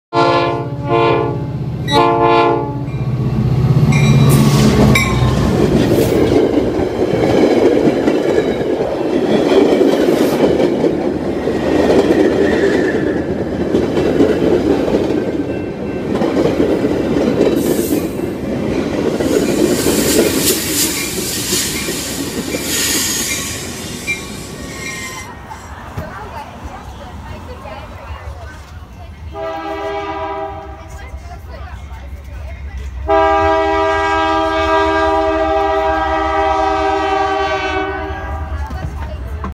Warbonnet on Mixed Freight in Louisville
A short clip of a Warbonnet in Louisville!  Lots of Warbonnet's have been coming through recently!